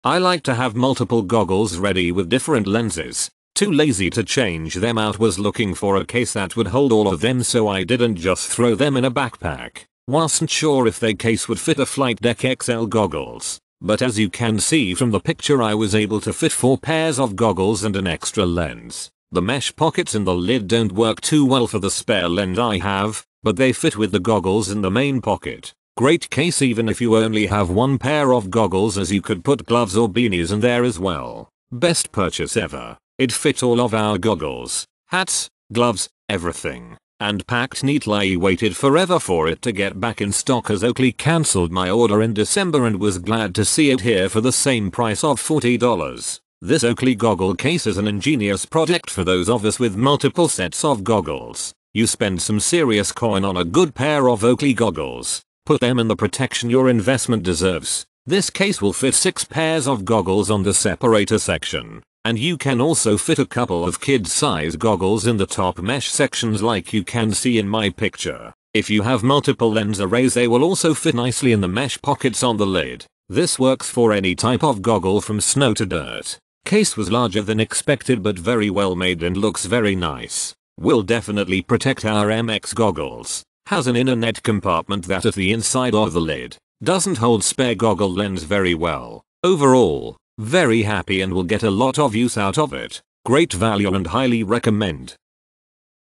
Oakley Multi Unit Goggles Case Black, Large review
Oakley Multi Unit Goggles Case Black, Large

Main Features:
- Nylon frame
- Padded nylon goggle storage
- Zip closure
- Carry handle